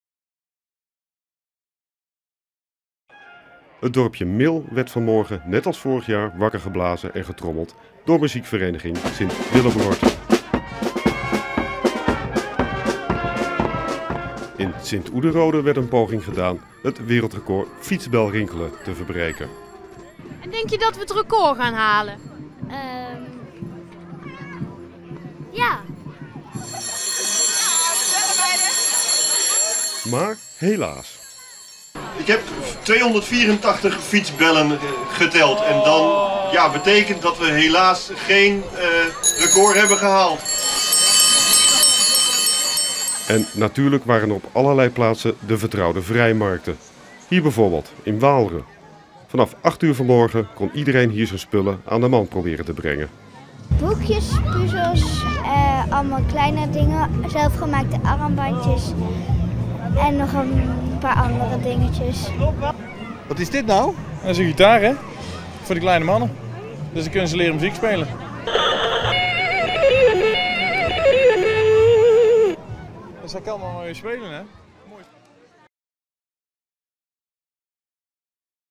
Koninginnedag in Brabant
Brabant viert Koninginnedag. De dag begint met regen, maar dat drukt de pret niet. De vrijmarkten, oud Hollandse spellen en de goede zin zorgt voor een geslaagde feestdag.